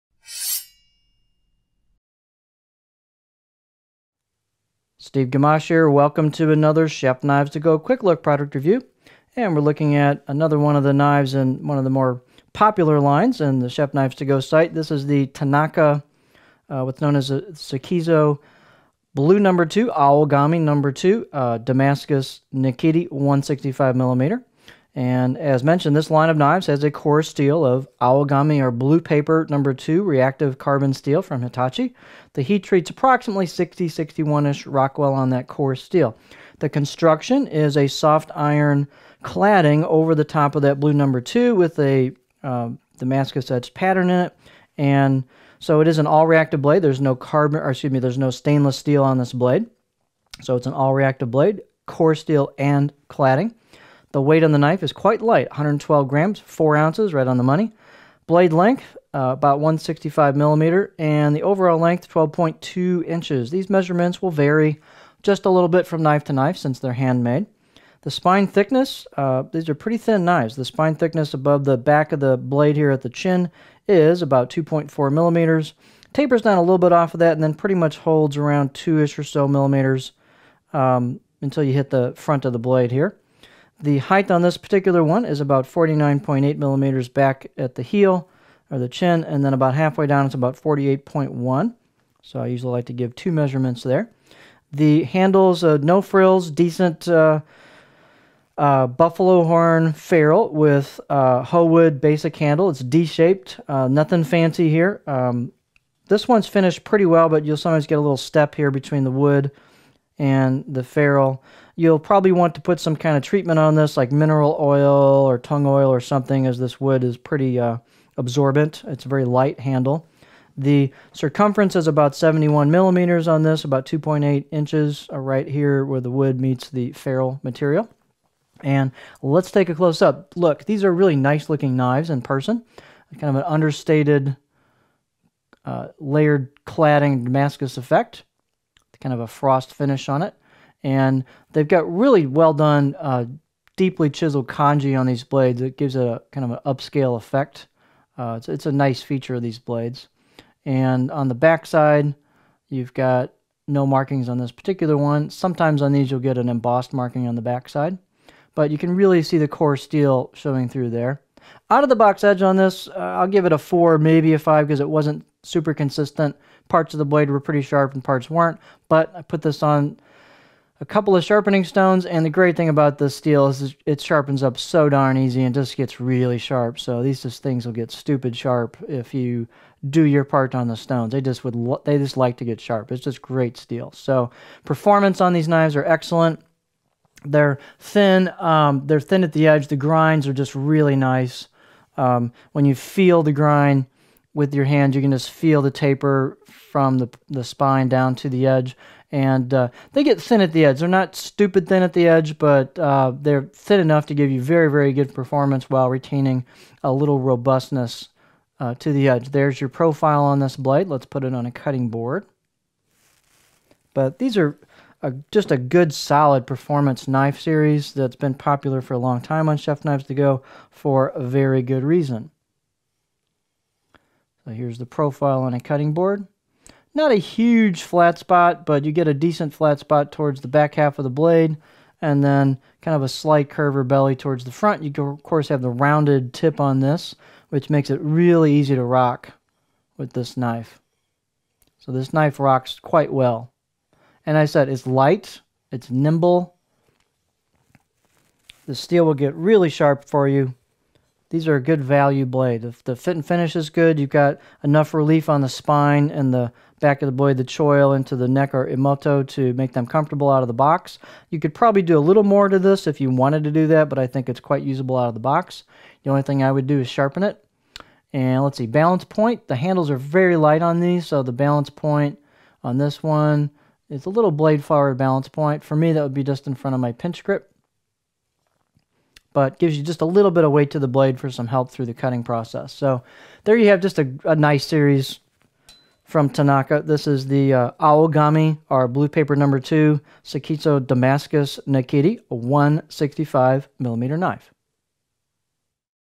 Tanaka B#2 Damascus Nakiri 165mm Quick Look
Here is a quick product demo of the Tanaka B#2 Damascus Nakiri 165mm.